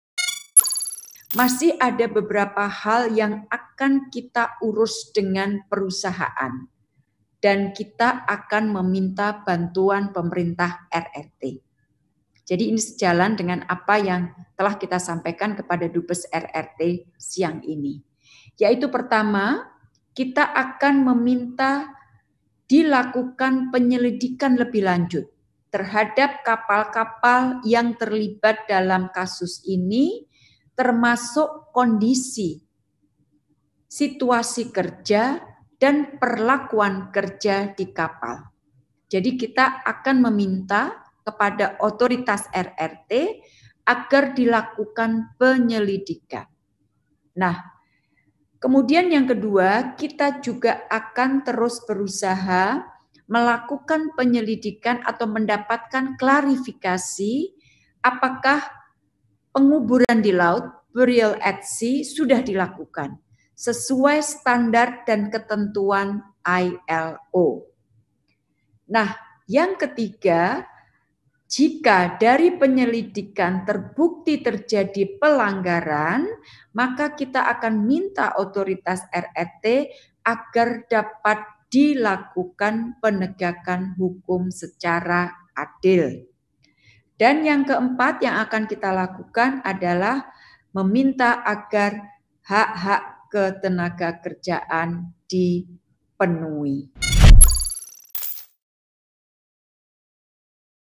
Kemlu Masih Selidiki Dugaan Eksploitasi ABK WNI di Kapal China!
Menteri Luar Negeri (Menlu) Retno Marsudi telah meminta bantuan pemerintah China untuk mengusut dugaan adanya eksploitasi anak buah kapal (ABK) Indonesia di kapal China.